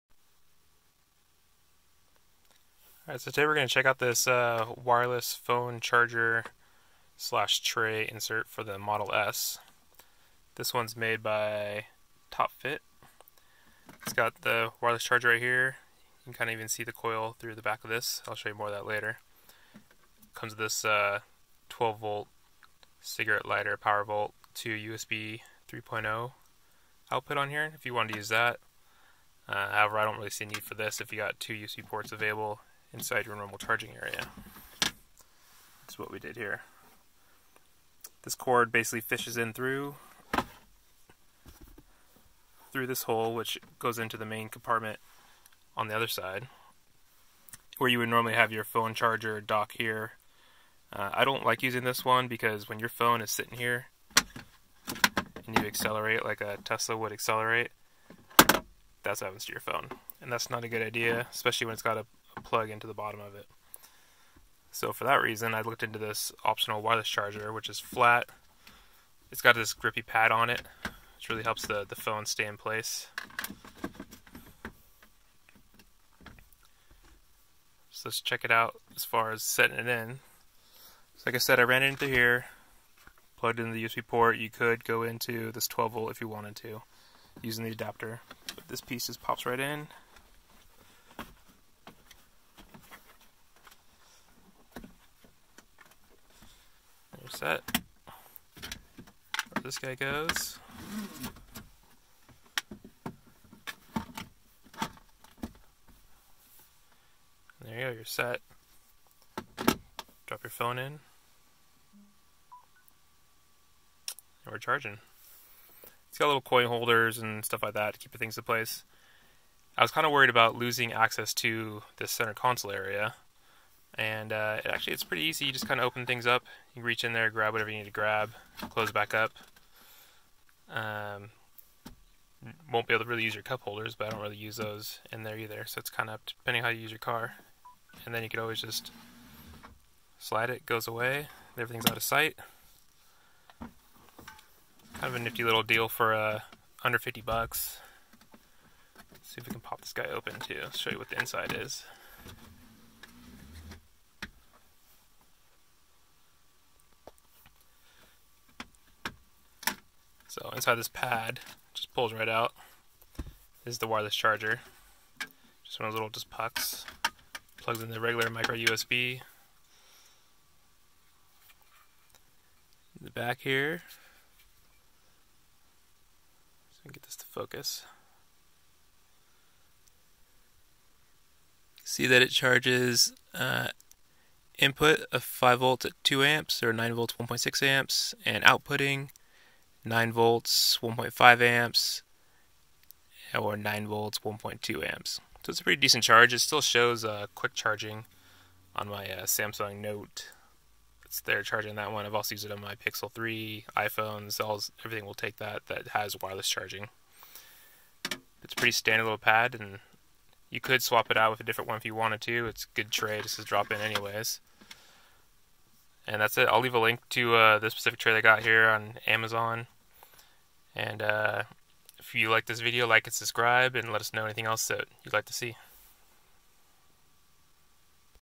Tesla Wireless Charger - Easy Drop In Accessory for Model S and X
Model S/X Wireless Charger

This super easy to install built in charger is compatible with all Qi-Enabled phones like, Google Pixel 3/3XL, iPhone X/8/8 plus, Samsung Galaxy S9/S9 Plus, and others.